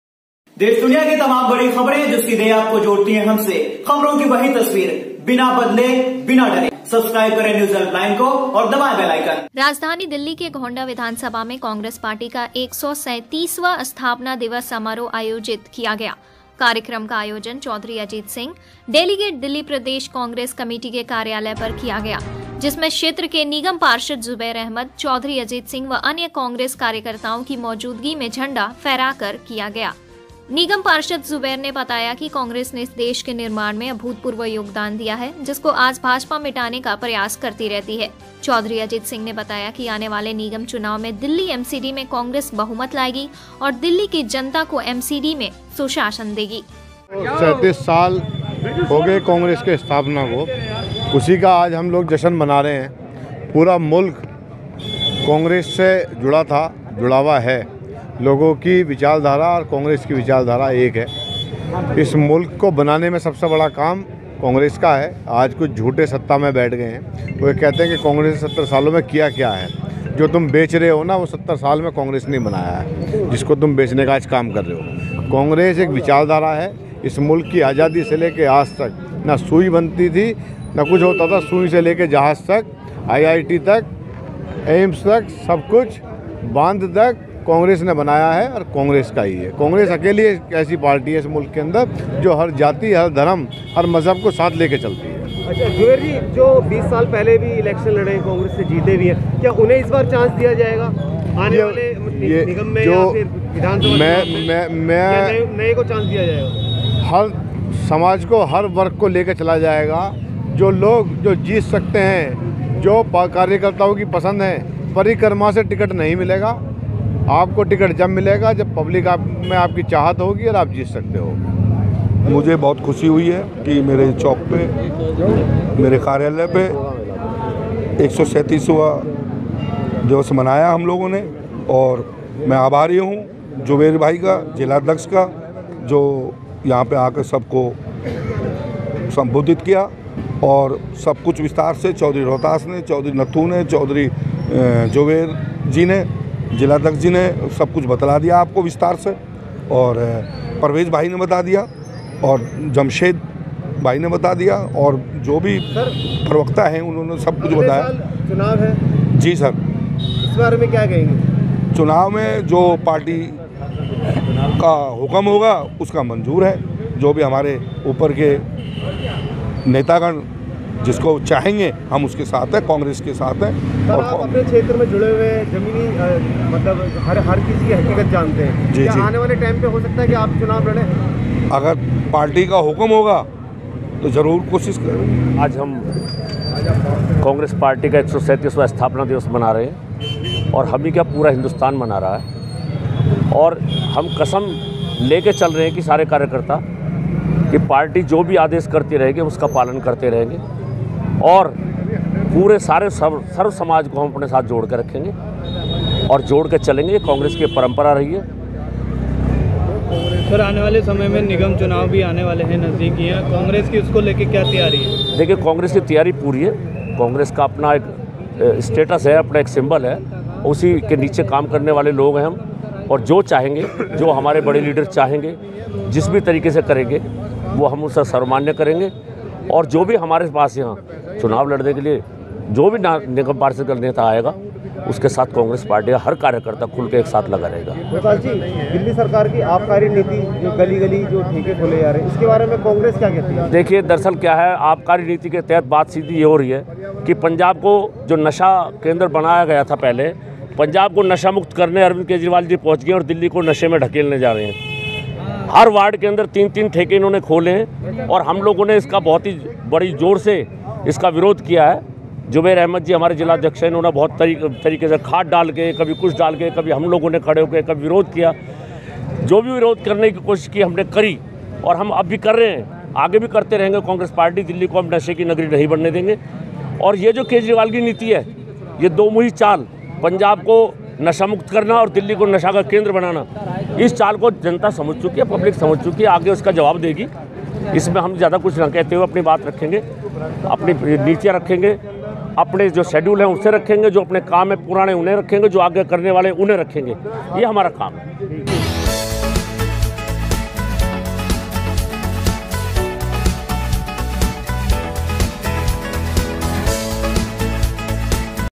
घौंडा विधानसभा में कांग्रेस पार्टी का 137 वां स्थापना दिवस समारोह का आयोजन किया गया || NewsHelpline
राजधानी दिल्ली के घौंडा विधानसभा में कांग्रेस पार्टी का 137 वां स्थापना दिवस समारोह आयोजित किया गया । कार्यक्रम का आयोजन चौधरी अजीत सिंह                      डेलीगेट दिल्ली प्रदेश कांग्रेस कमेटी के कार्यालय पर किया गया । जिसमें क्षेत्र के निगम पार्षद ज़ुबैर अहमद , चोधरी अजीत सिंह व अन्य कांग्रेस कार्यकर्ताओं की मौजूदगी में झण्डा फ़ेरा कर किया गया ।
निगम पार्षद ज़ुबैर ने बताया कि कांग्रेस ने इस देश के निर्माण में अभूतपूर्व योगदान दिया है जिसको आज भाजपा मिटाने का प्रयास करती रहती है । चौधरी अजीत सिंह ने बताया कि  आने वाले निगम चुनाव में दिल्ली mcd में कांग्रेस बहुमत लाएगी और दिल्ली की जनता को mcd में सुशासन देगी ।